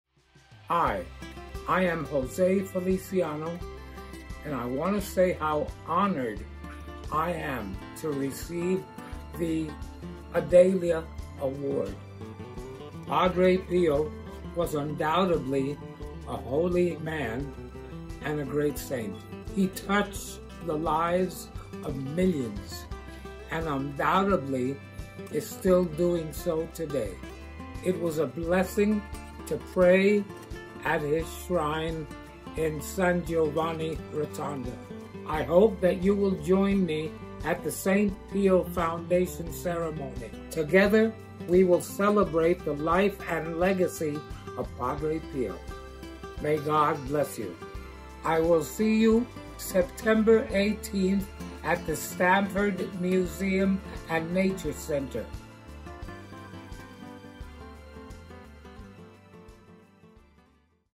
Grammy award artist Jose Feliciano speech in receiving the Adelia Award
Grammy award artist and international singer Jose Feliciano acceptance speech in receiving the Saint Pio Award on September 18, 2021, at the Stamford Museum and Nature Center in Stamford, CT